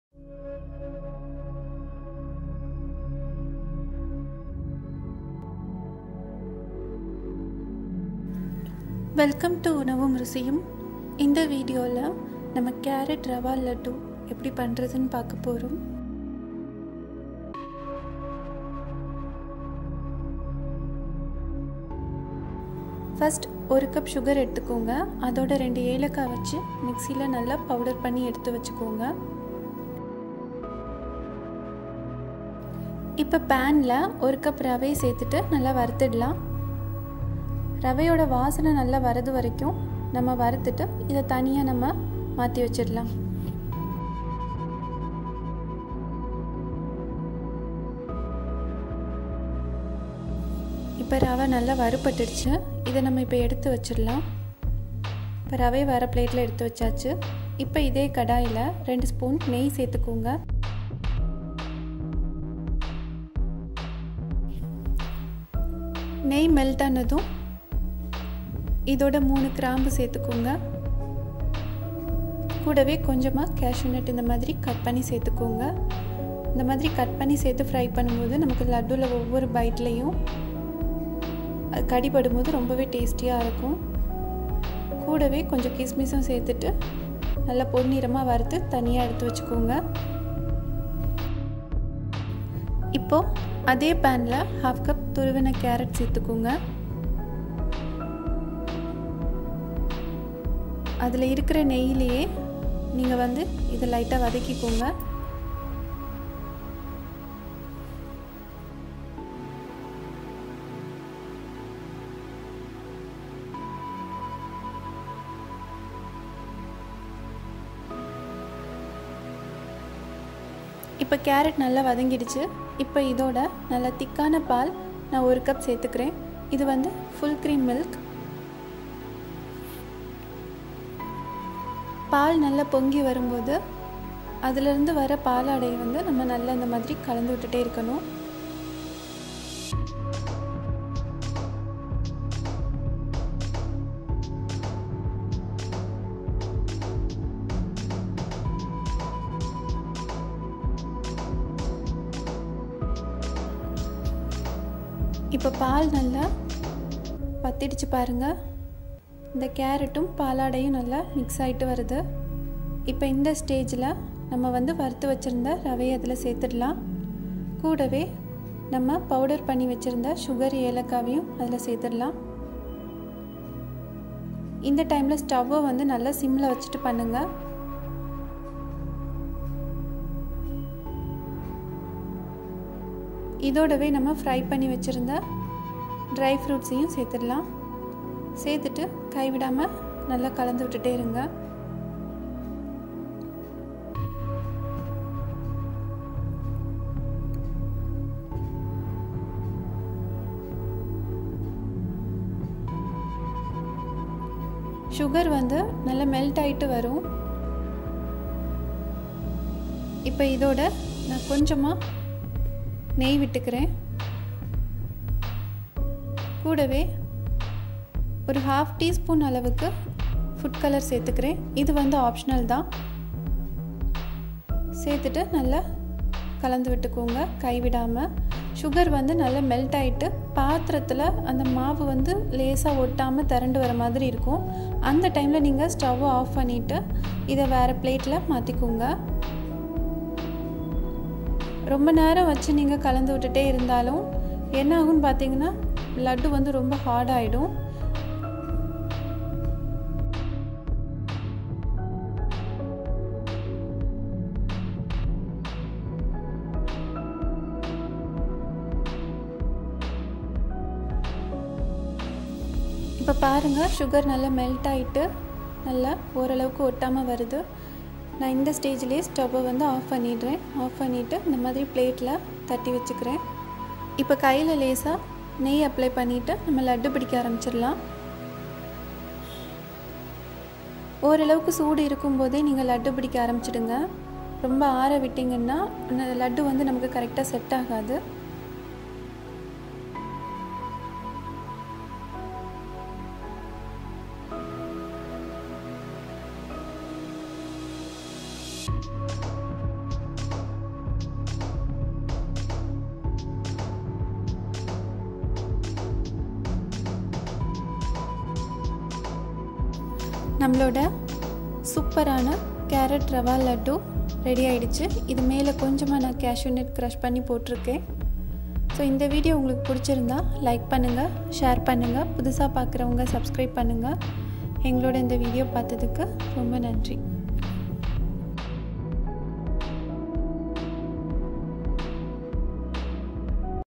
Carrot Rava Laddu / கேரட் ரவா லட்டு (Diwali special)
Carrot Rava Laddu / கேரட் ரவா லட்டு

ingredients:

Ghee
Cashew nut
Kismis
Cardamom - 2
Clove - 3